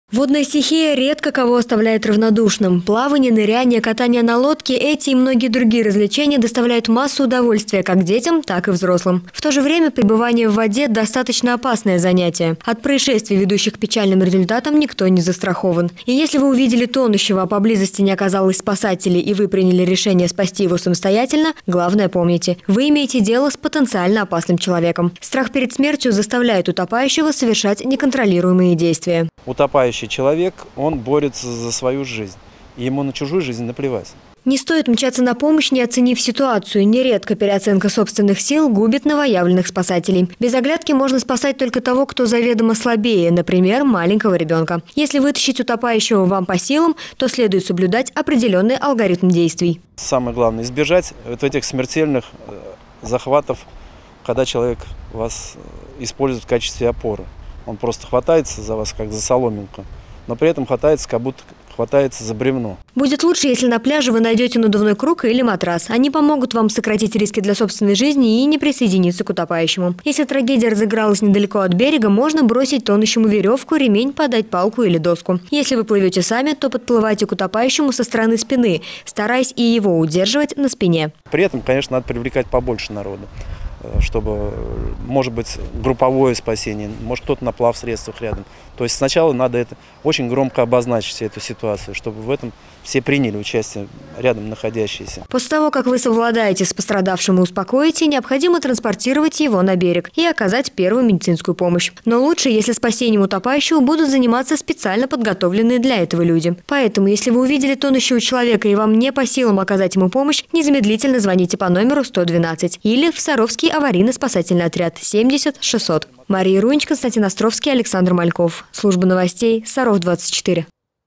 Правила спасения утопающего
Специалисты МЧС напоминают ряд правил, которые помогут вам не только спасти чужую жизнь, но и не погибнуть самому.